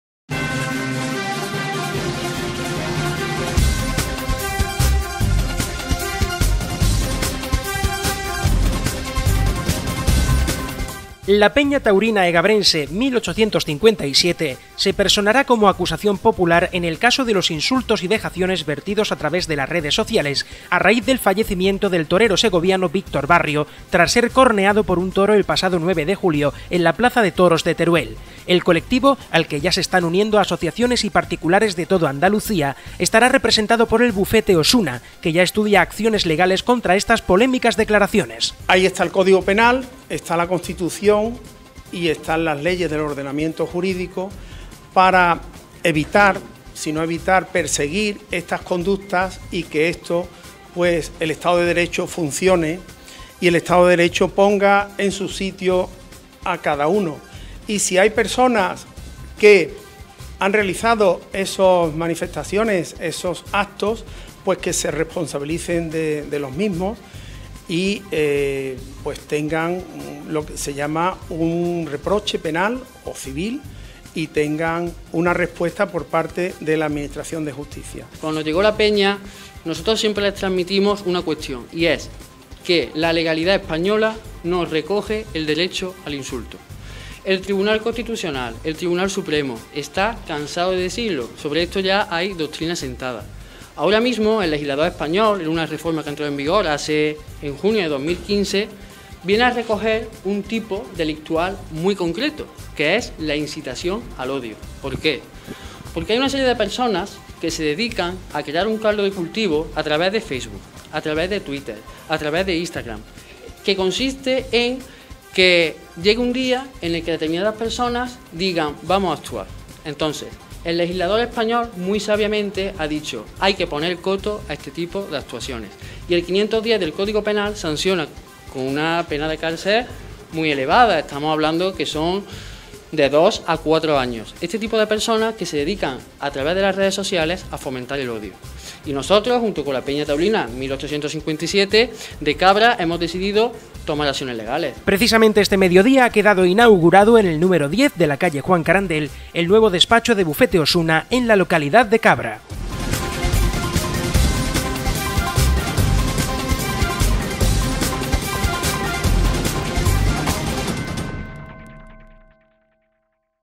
CABRA - La PeñaTaurina1857‬ demanda por la muerte de Victor Barrio, 18/07/2016
‪CABRA‬ - La ‪PeñaTaurina1857‬ emprenderá acciones legales contra los ‪insultos‬ en las ‪redessociales‬ tras la ‪muerte‬ del ‪‎torero‬ ‪‎VictorBarrio‬. Estarán representados por ‪BufeteOsuna‬ que hoy ha inaugurado su nuevo ‪‎despacho‬ en la localidad.